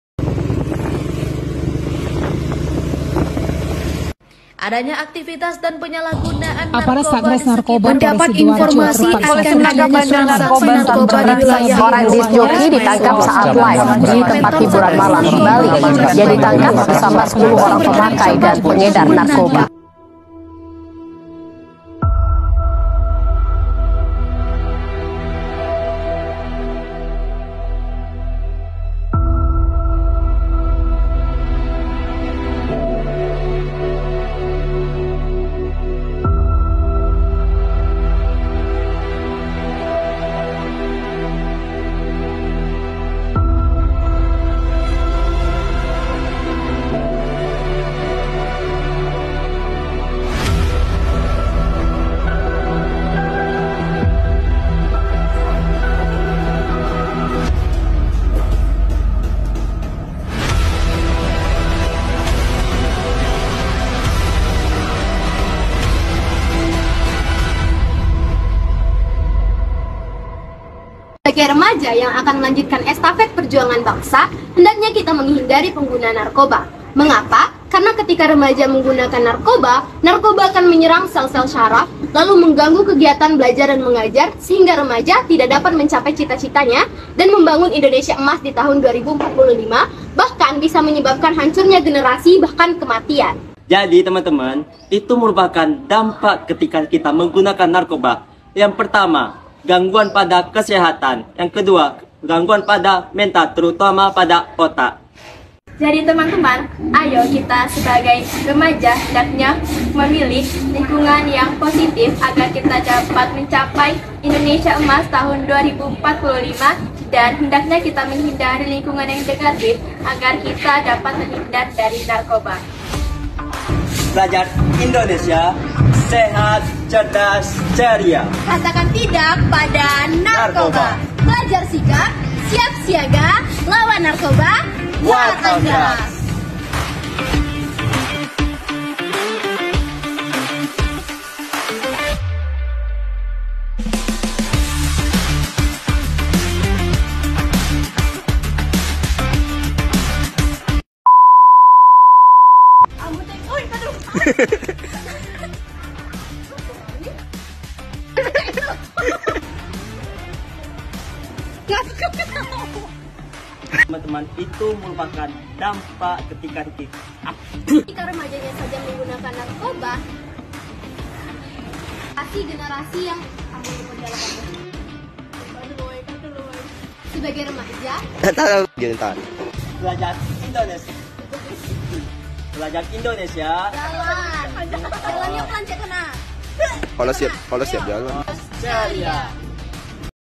WAR ON DRUGS
‼️WARNING‼️
Semua adegan dan tempat dalam video ini hanya ilustrasi dan akting belaka. Bukan adegan/kejadian asli dan tidak untuk ditiru.

Sebuah karya film pendek tanpa dialog dengan edukasi di dalamnya. Menceritakan tentang seorang pelajar yang melakukan transaksi narkoba dan menjadi "pemakai".

Hendaknya sebagai remaja yang menjadi ujung tombak majunya suatu bangsa dan akan melanjutkan estafet perjuangan bangsa, kita menghindari penyalahgunaan narkoba demi mewujudkan 𝗜𝗻𝗱𝗼𝗻𝗲𝘀𝗶𝗮 𝗘𝗺𝗮𝘀 𝟮𝟬𝟰𝟱.

𝐀𝐲𝐨 𝐩𝐞𝐫𝐚𝐧𝐠𝐢 𝐧𝐚𝐫𝐤𝐨𝐛𝐚, 𝐝𝐞𝐦𝐢 𝐦𝐚𝐬𝐚 𝐝𝐞𝐩𝐚𝐧 𝐈𝐧𝐝𝐨𝐧𝐞𝐬𝐢𝐚‼️